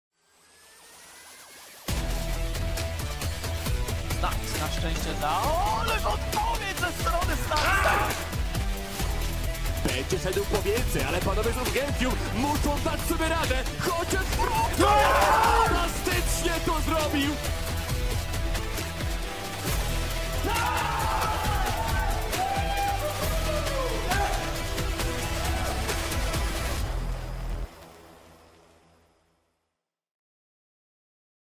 Wiosenny sezon ESL Mistrzostw Polski coraz bliżej!
adchodzi 24. sezon ESL Mistrzostw Polski! 🇵🇱🏆

🔫 12 drużyn
🆎 2 grupy
💰 100 000 PLN
🔝 Awans do ESLProLeague Conference

Zapisz się na turnieje ESL!